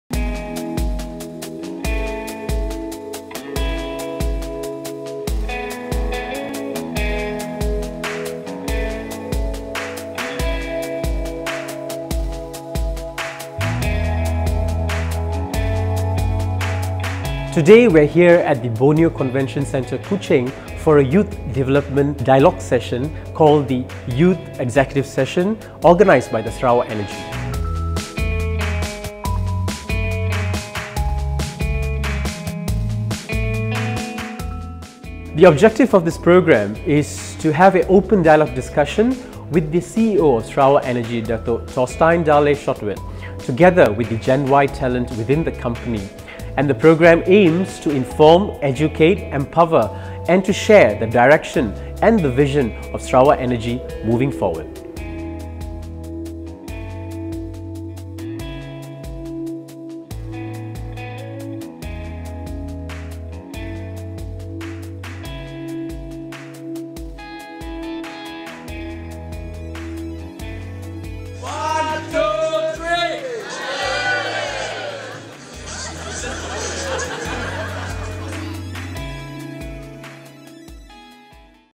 A Focus on Sarawak Energy's Youths
Sarawak Energy’s Young Executive Session (YES) serves as a platform for open dialogue between Group Chief Executive Officer Datuk Torstein Dale Sjotveit and the company's Gen-Y talents.